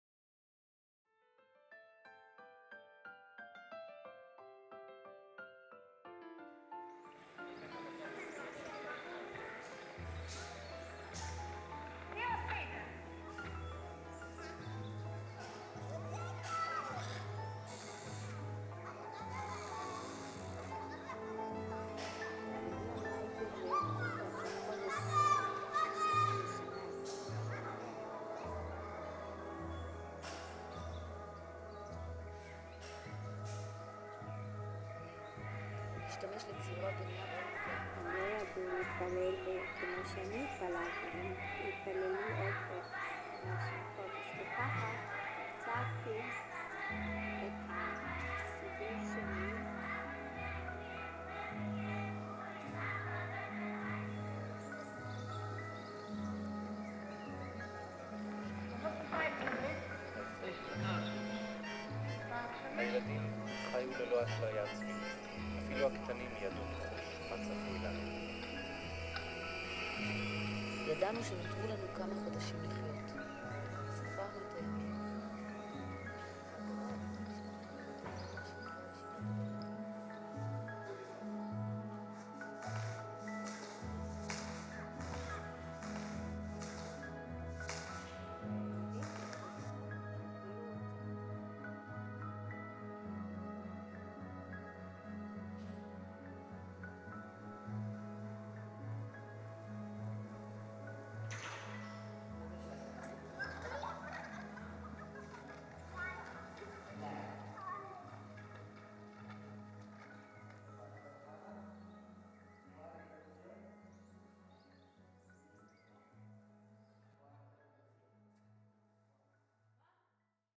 Virtual Tour of Children's Exhibition at Yad Vashem
The new exhibition "Children in the Holocaust – Stars Without a Heaven"  gives expression to the lives of the 1.5 million children who were murdered in the Holocaust.  It displays the personal stories that remained of these children or the stories of the few who survived.

"Children in the Holocaust – Stars Without a Heaven"
ילדות בשואה – כוכבים בלי שמים""